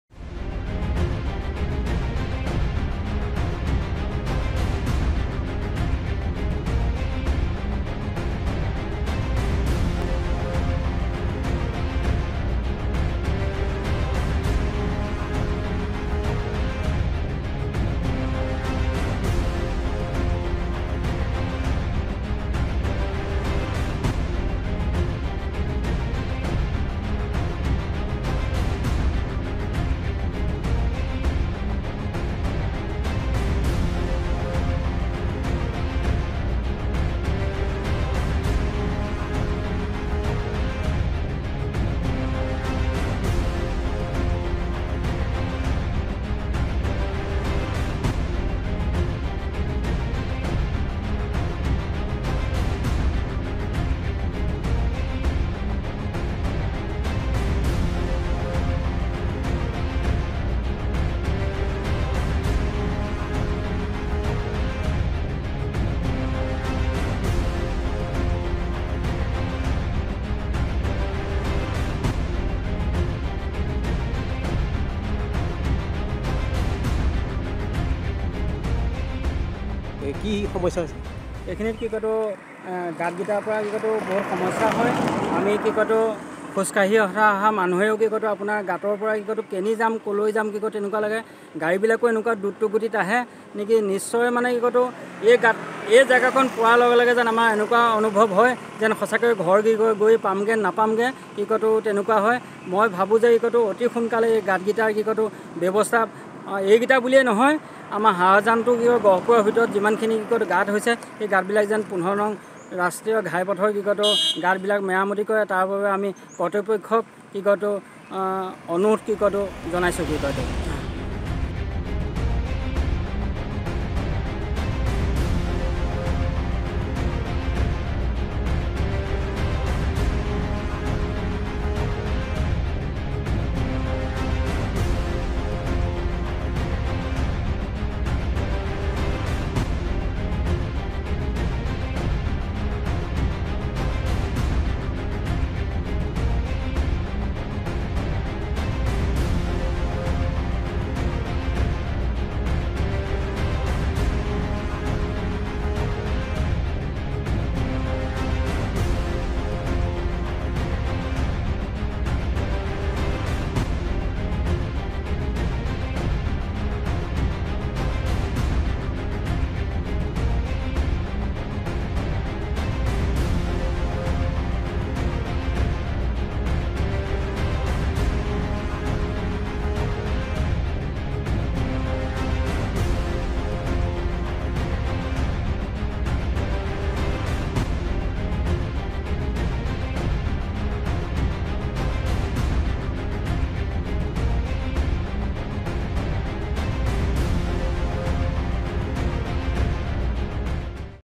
বিশ্বনাথ জিলাৰ গহপুৰৰ ১৫ নং ৰাষ্ট্ৰীয় ঘাইপথৰ চেংমৰাত আছে এটা ACCIDENT ZONE. চাওঁক এক প্ৰতিবেদন
বিশ্বনাথ জিলাৰ গহপুৰৰ ১৫ নং ৰাষ্ট্ৰীয় ঘাইপথৰ চেংমৰাত আছে এটা ACCIDENT ZONE
য’ত পথচাৰীৰ বাবে বিৰাজমান আতংকৰ
চাওঁক এক প্ৰতিবেদন